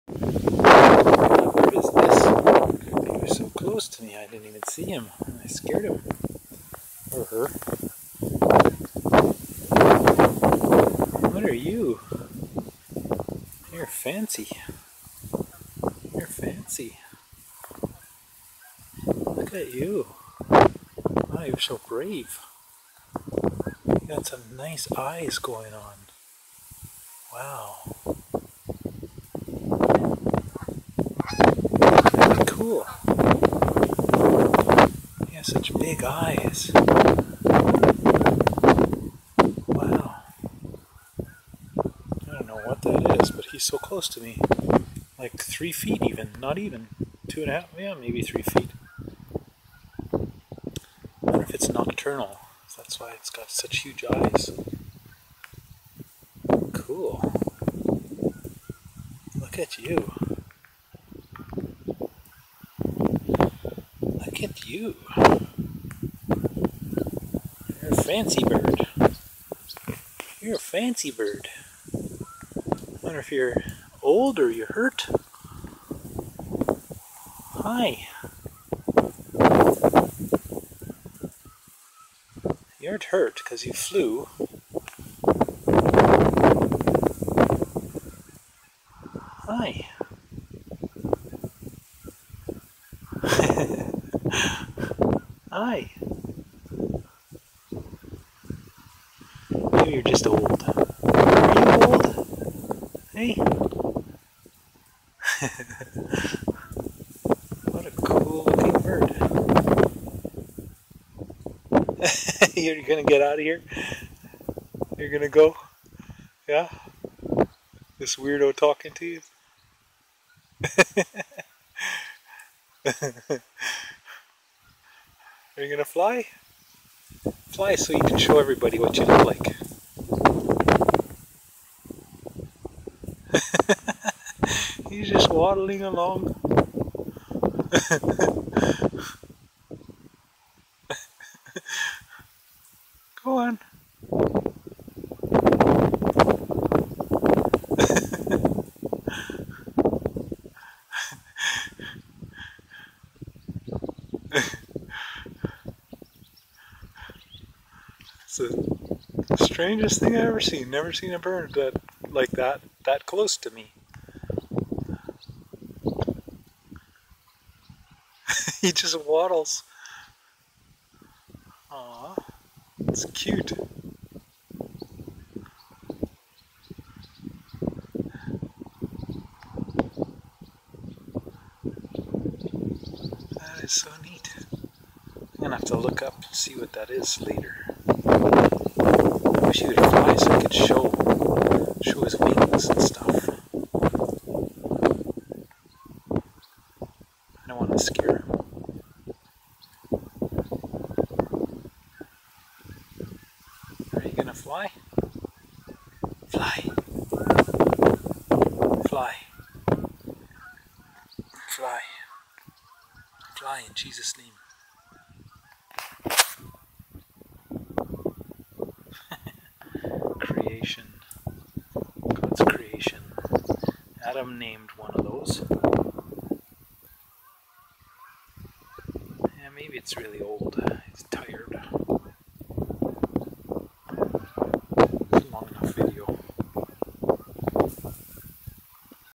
Common Nighthawk hanging out and waddling at Oak Hammock Marsh
09092016 Oak Hammock Marsh, a common nighthawk sits atop a rail for a wooden boardwalk through the marsh. Very cute female nighthawk. Enjoy!